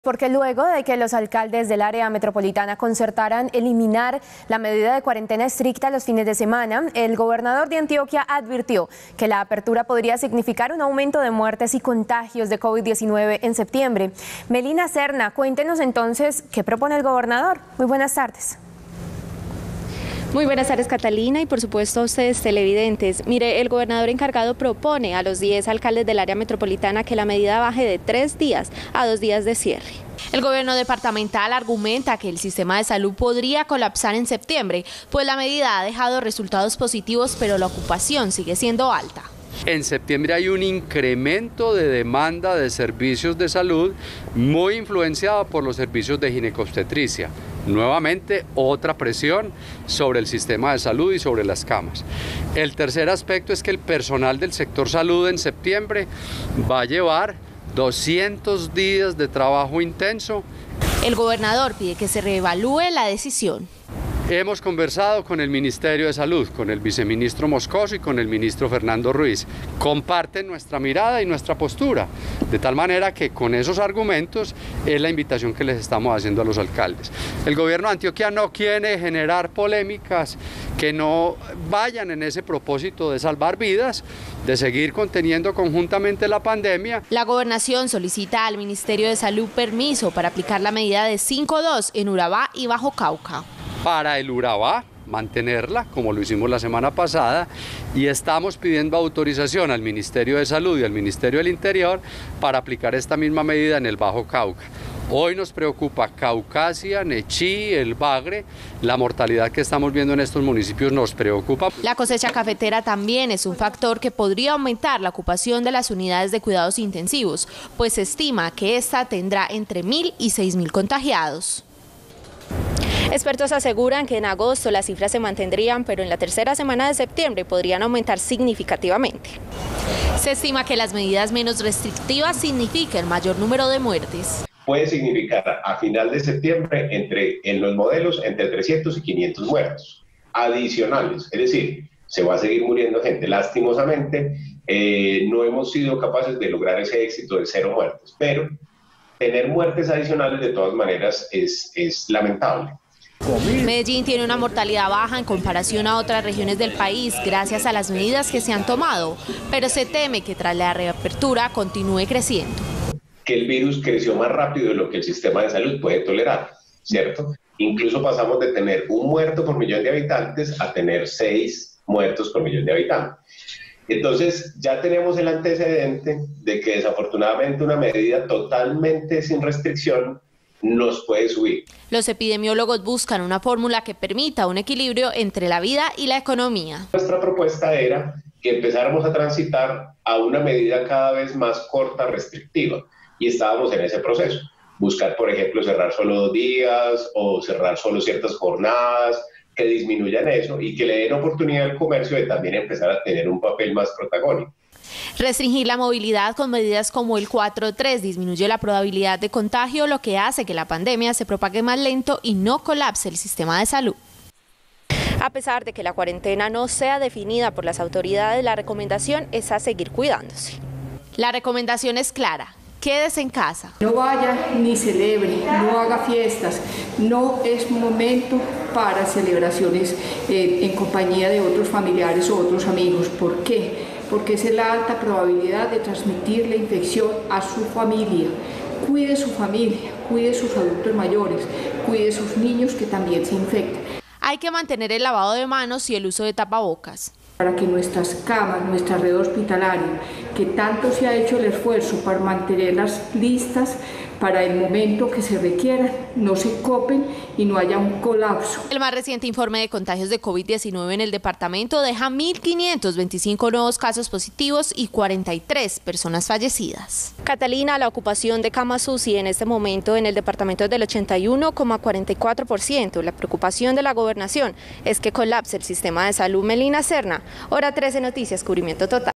Levantamiento de cuarentenas estrictas aumentarían contagios y muertes por covid-19
Luego de que los alcaldes del Área Metropolitana concertaran eliminar la medida de cuarentena estricta los fines de semana, el gobernador de Antioquia advirtió que la apertura podría significar un aumento de muertes y contagios de covid-19 en septiembre.